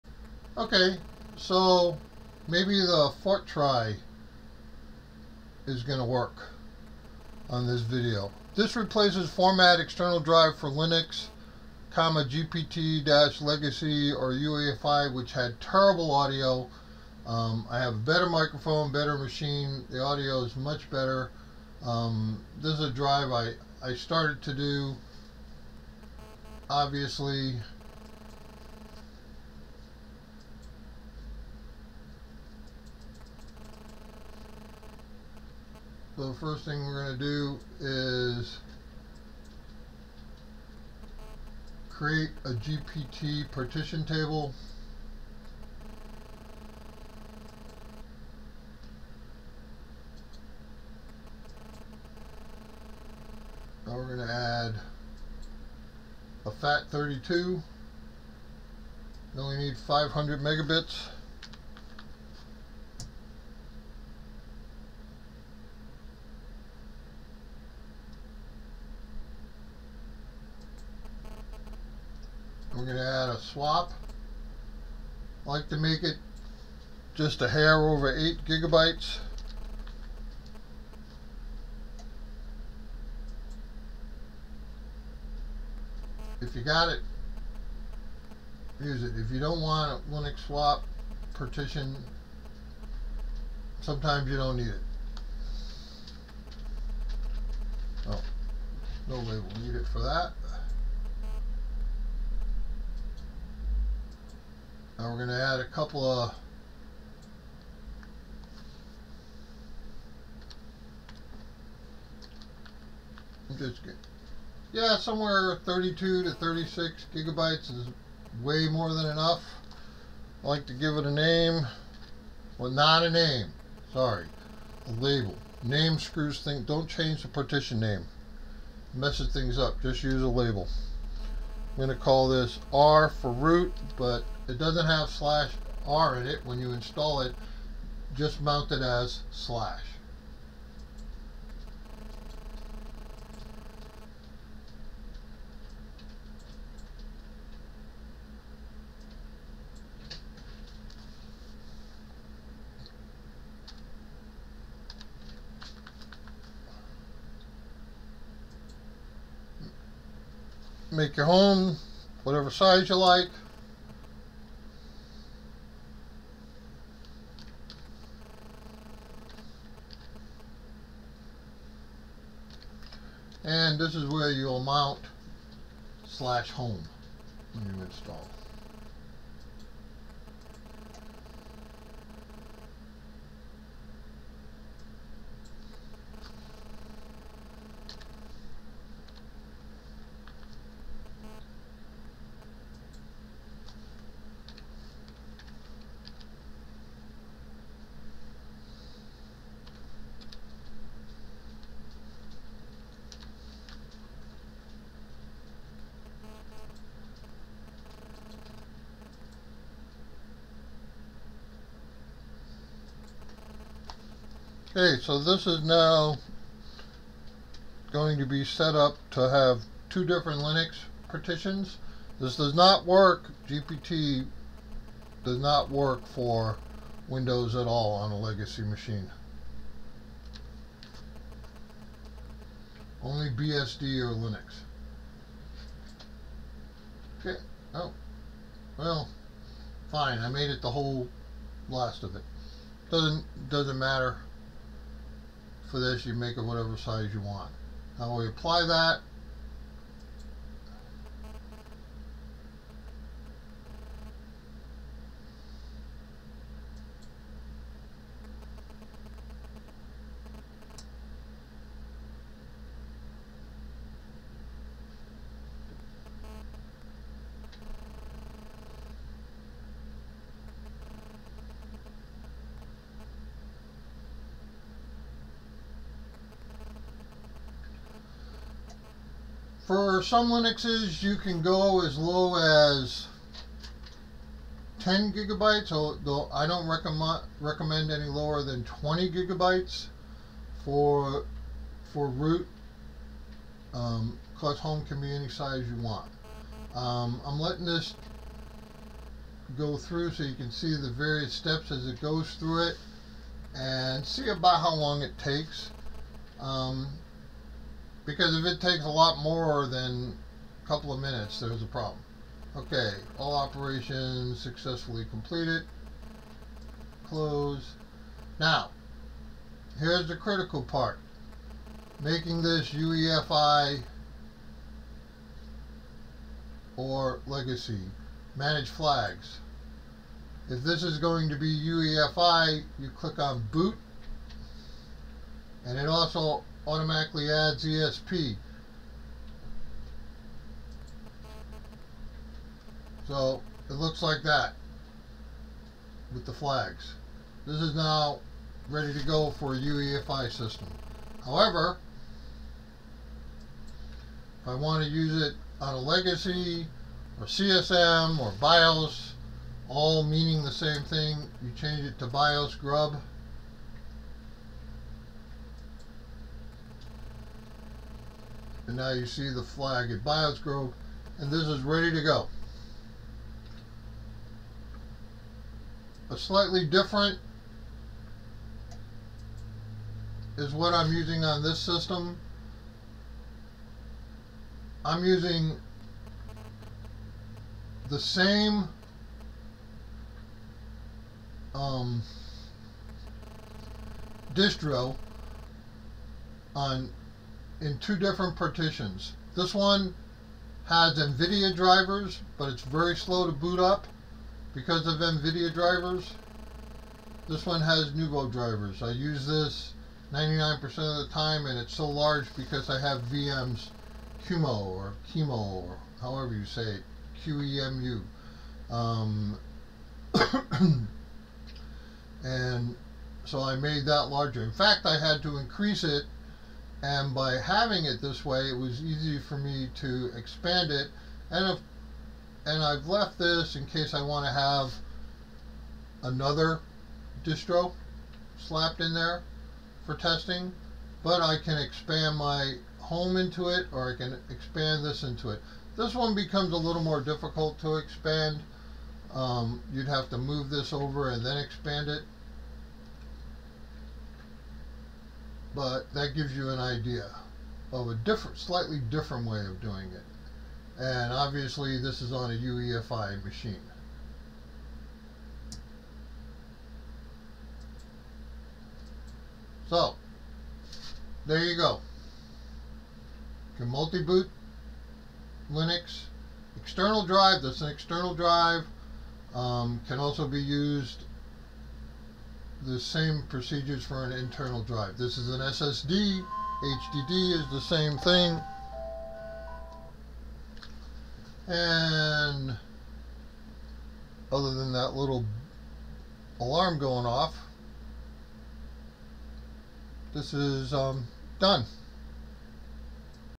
Partition Linux drive with GPT for Bios or UEFI - Improved Audio
You can use gpt for Linux on may BIOS only machines. This shows you how.